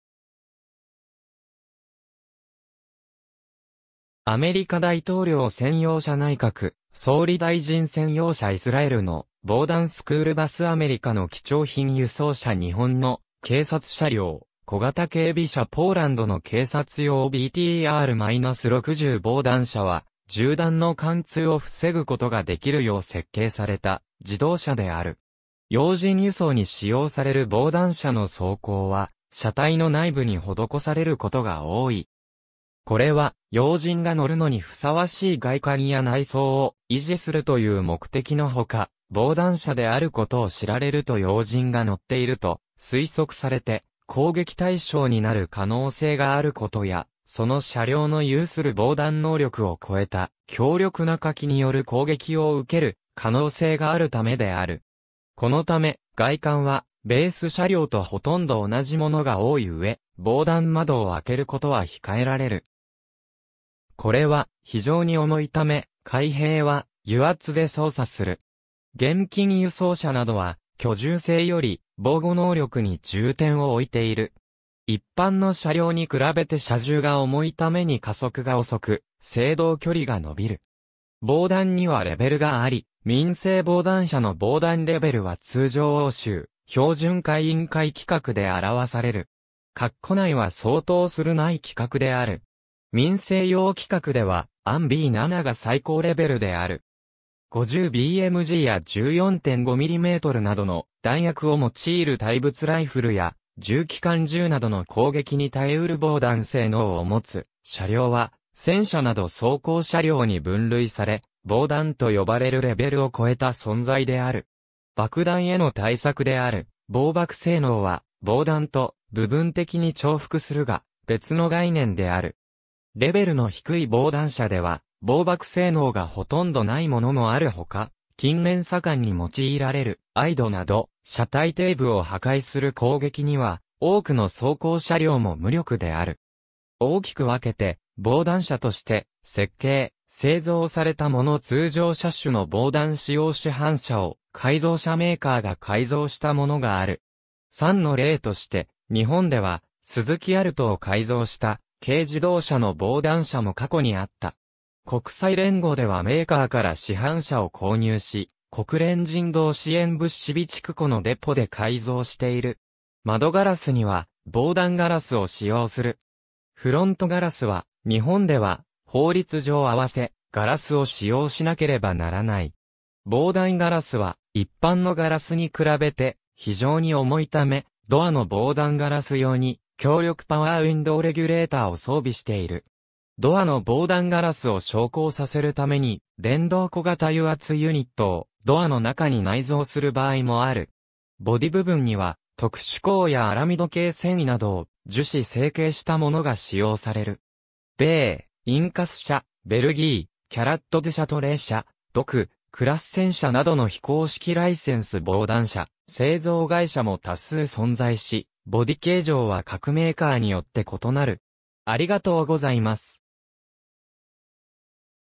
防弾車
装甲_(車両)
アメリカ大統領専用車 内閣総理大臣専用車 イスラエルの防弾スクールバス アメリカの貴重品輸送車 日本の警察車両「小型警備車」 ポーランドの警察用BTR-60（軍用車両の警察仕様） 防弾車（ぼうだんしゃ）は、銃弾の貫通を防ぐことができるよう設計された自動車である。
要人輸送に使用される防弾車の装甲は車体の内部に施されることが多い。
これは、要人が乗るのにふさわしい外観や内装を維持するという目的のほか、防弾車であることを知られると要人が乗っていると推測されて攻撃対象になる可能性があることや、その車両の有する防弾能力を超えた強力な火器（ロケットランチャーや重機関銃など）による攻撃を受ける可能性があるためである。
このため、外観はベース車両とほとんど同じものが多いうえ、防弾窓を開けることは控えられる。
これは非常に重いため、開閉は油圧で操作する。
現金輸送車などは、居住性より防護能力に重点を置いている。
一般の車両に比べて車重が重いため（通常車の約2-3倍の4-5トン）に加速が遅く、制動距離が伸びる。
防弾にはレベルがあり、民生防弾車の防弾レベルは通常欧州標準化委員会 (CEN) 規格で表される。
括弧内は相当するNIJ規格（防弾ベストなどに使われる）である。
民生用規格ではEN-B7が最高レベルである。
.50 BMGや14.5mmなどの弾薬を用いる対物ライフルや重機関銃などの攻撃に耐え得る防弾性能を持つ車両は、戦車など装甲車両に分類され、「防弾」と呼ばれるレベルを超えた存在である（装甲兵員輸送車の多くも、こうした火器への防御は完璧ではない）。
爆弾（特に地雷）への対策である防爆性能（可燃性液体、あるいは気体の存在する場所で使用される電気機器に求められる「爆発を防ぐ機能」としての防爆規格とは別のものである）は、防弾と部分的に重複するが、別の概念である。
レベルの低い防弾車では防爆性能がほとんどないものもあるほか、近年盛んに用いられるIEDなど車体底部（一般的に装甲が薄く脆弱である）を破壊する攻撃には、多くの装甲車両も無力である。
大きく分けて、 防弾車として設計・製造されたもの 通常車種の防弾仕様 市販車を改造車メーカーが改造したもの がある。
3の例として、日本ではスズキ・アルトを改造した軽自動車の防弾車（NIJ-IIIA）も過去にあった。
国際連合ではメーカーから市販車を購入し、国連人道支援物資備蓄庫のデポで改造している。
窓ガラスには、防弾ガラスを使用する。
フロントガラスは、日本では法律上合わせガラスを使用しなければならない。
防弾ガラスは一般のガラスに比べて非常に重いため、ドアの防弾ガラス用に強力パワーウインドウレギュレーターを装備している。
ドアの防弾ガラスを昇降させるために、電動小型油圧ユニットをドアの中に内蔵する場合もある。
ボディ部分には、特殊鋼やアラミド系繊維などを樹脂成形したものが使用される。
米「インカス社」、ベルギー「キャラットドゥシャトレ社」、独「クラッセン社」（クラッセン社のロシアでの防弾車製造は「rida社」が行っている）などの非公式ライセンス防弾車製造会社も多数存在し、ボディ形状（車高や延長具合、内装の装飾など）は各メーカーによって異なる。